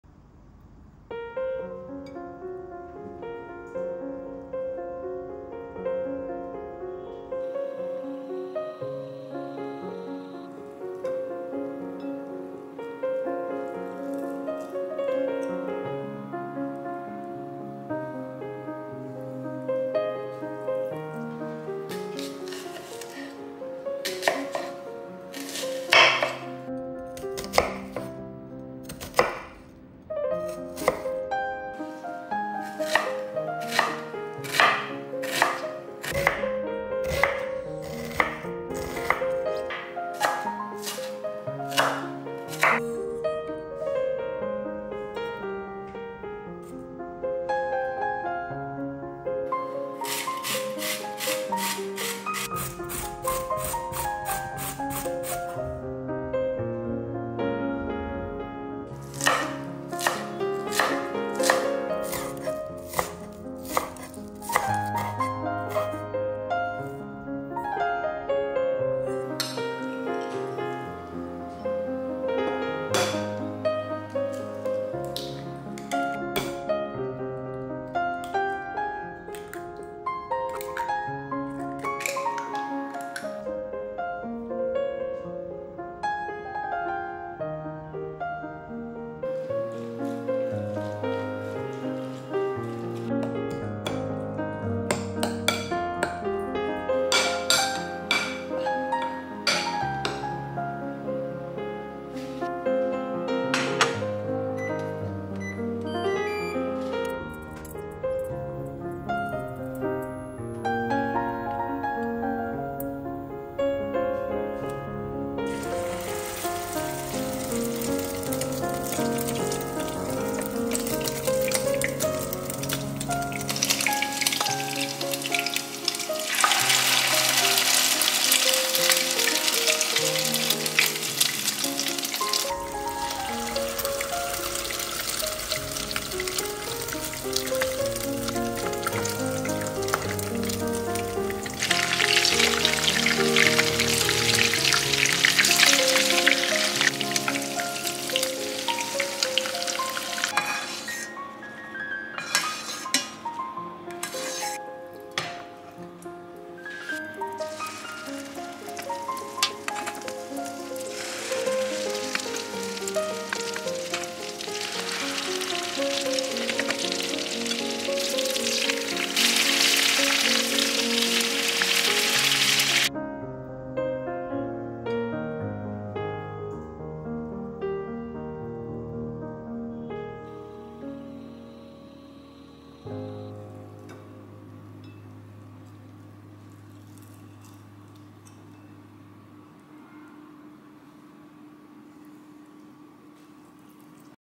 Crispy Pork-Egg Cakes: It takes only 10 Minutes and 2 Key Ingredients
The combination of ground pork and eggs along with onion and carrots creates a delicious dish that is well-seasoned, moist, and slightly crispy with a rich and savory taste. Pork-Egg cakes are commonly served with rice in many Asian cuisines but you may also serve them with bread or other types of carbohydrates, depending on your personal preferences. Guess what, it takes you only 10 minutes with as minimum as 2 ingredients. Enjoy!